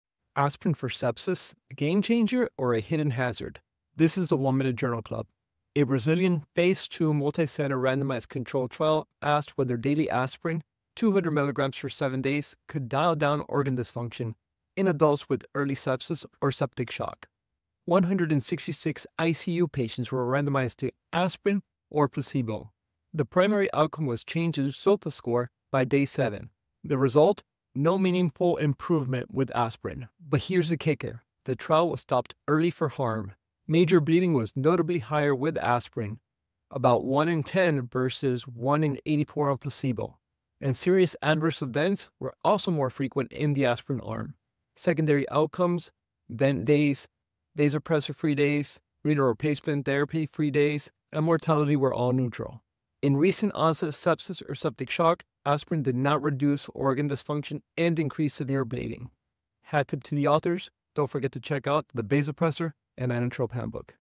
Aspirin in Sepsis? RCT Says “Not So Fast” (↑ Bleeding, No SOFA Benefit)
Today, we discuss a Brazilian phase two multicenter randomized controlled trial examining the effects of daily 200 mg aspirin for seven days on adults with early sepsis or septic shock. The trial, which involved 166 ICU patients, was stopped early due to increased major bleeding in the aspirin group. No meaningful improvement in the primary outcome (change in SOFA score by day seven) was observed, and secondary outcomes such as ventilator days, vasopressor-free days, renal replacement therapy-free days, and mortality remained neutral. Find out more about the implications of these findings in sepsis treatment.

Citation: Almeida TML, Freitas FGR, Figueiredo RC, Houly SG, Azevedo LCP, Cavalcanti AB, Damiani LP, Svicero BS, Souza MA, Bustamante CLS, Ramos FJS, Alves RSV, Atallah FC, Jackiu M, Pacheco ES, Schmidt RC, Serra FSC, Tomotani DYV, Zampieri FG, Machado FR; BRICNET. Acetylsalicylic Acid Treatment in Patients With Sepsis and Septic Shock: A Phase 2, Placebo-Controlled, Randomized Clinical Trial. Crit Care Med. 2025 Feb 1;53(2):e269-e281. doi: 10.1097/CCM.0000000000006564. Epub 2025 Feb 21. PMID: 39982179.